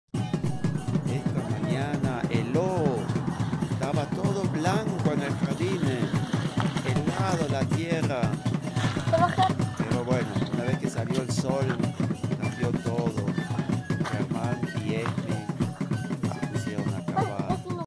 Ger y Esme jugando con agua y tierra helada
Esta mañana cayo una helada. Ger y Esme se pusieron a jugar con Alegria una vez que salio el Sol!!!☺